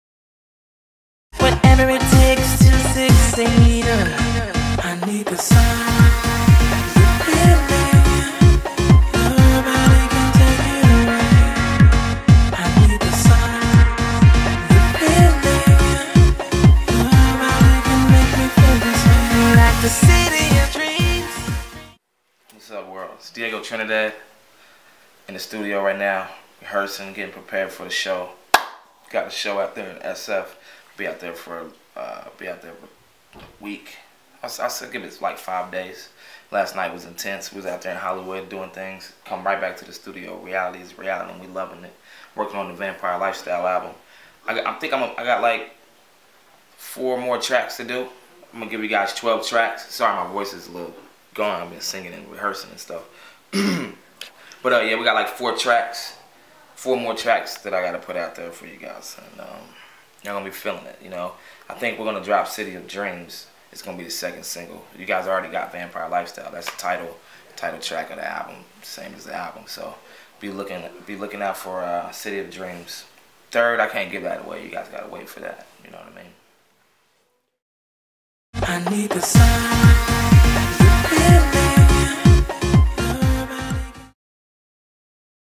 InTheStudio - Diego Trinidad - Front Kick Review
Front Kick Review was behind the scenes during the recording of Diego Trinidad's new song. If you want to see more of Diego Trinidad, check out his youtube page or follow him on twitter @DiegoTrinidad